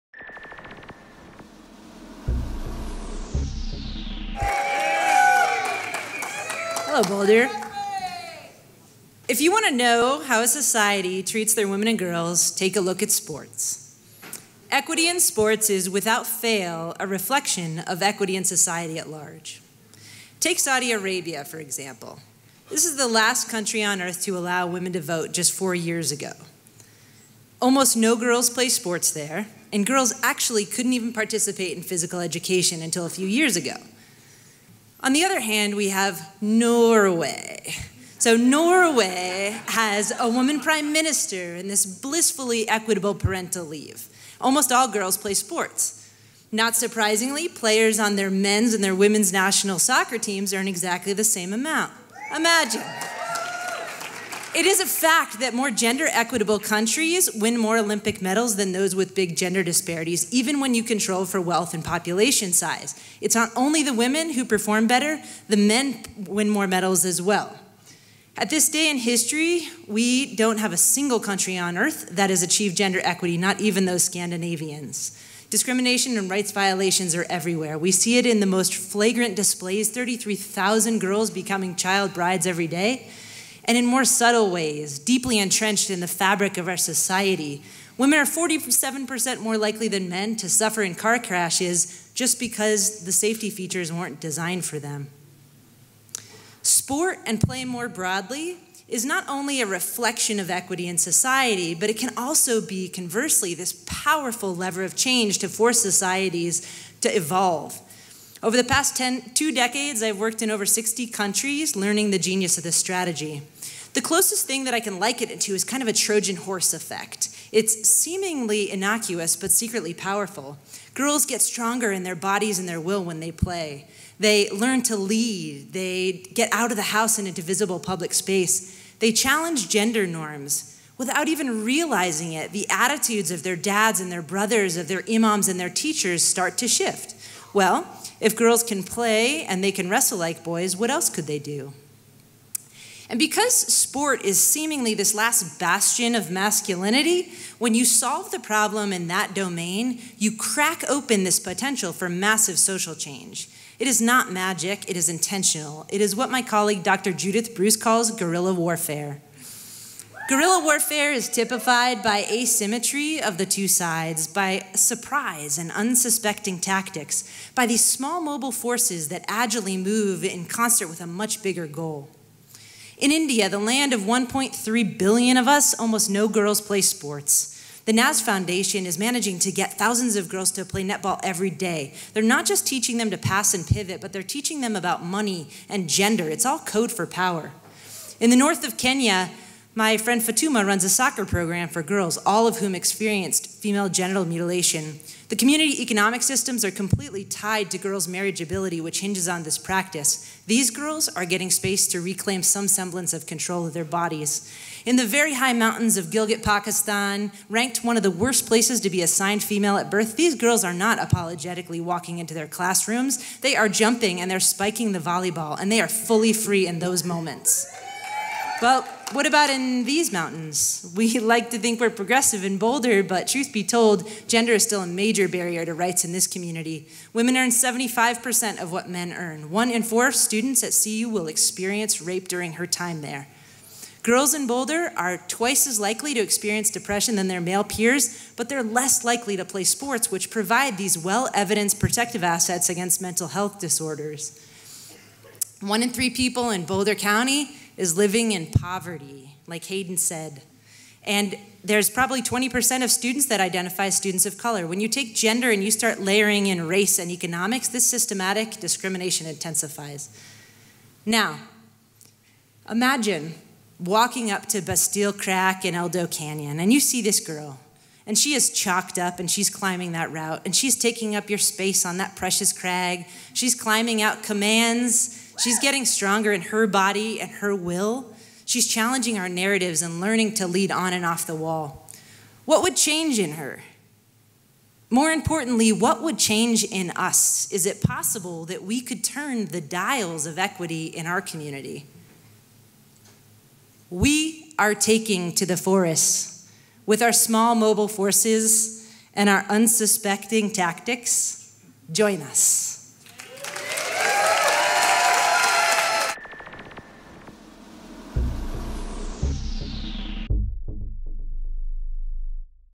Sarah Murray "A Guerrilla Strategy for Transforming Gender Equity in Society" at Ignite Boulder 40
Sport is considered by some as the last place it's acceptable to discriminate against women and girls. Sarah Murray shares insight garnered from work in over 60 countries about how play can be a most potent weapon in the fight against gender inequity globally and at home in Boulder, Colorado.